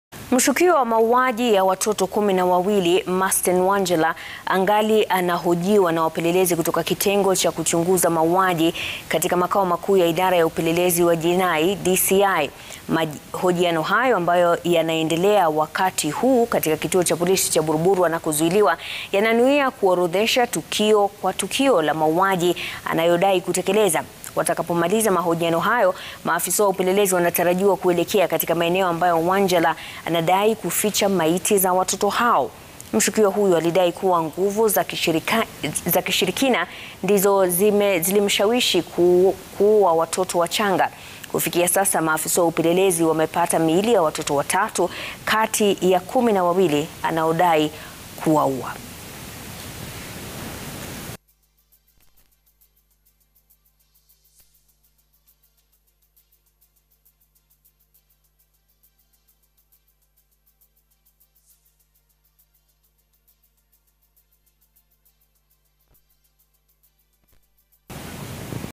Mshukiwa wa mauji ya watoto Masten Wanjala ahojiwa na wapelelezi
Wanjala alikiri kuwauwa watoto 12 na kuonesha alikotupa miili mitatu
Mahakama iliruhusu polisi wamzulie kwa siku 30 wakamilishe uchunguzi